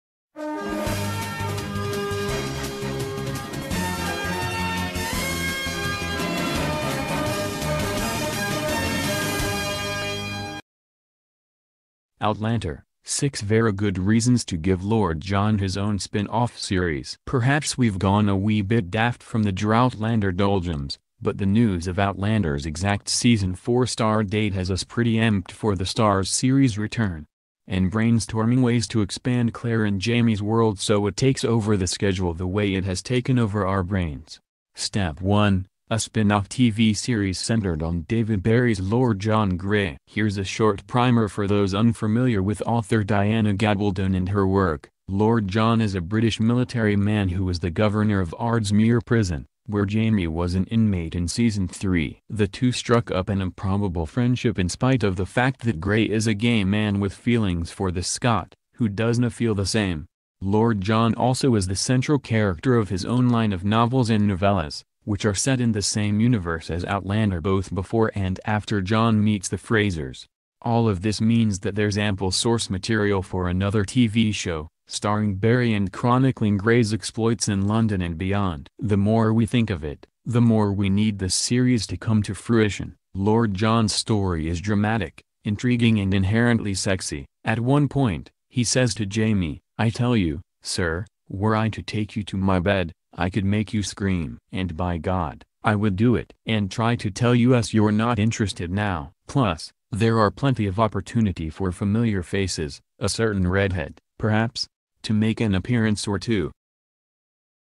'Outlander': Reasons to Have a Lord John Spinoff TV Series
Outlander 6 Verra Good Reasons to Give Lord John His Own Spinoff Series.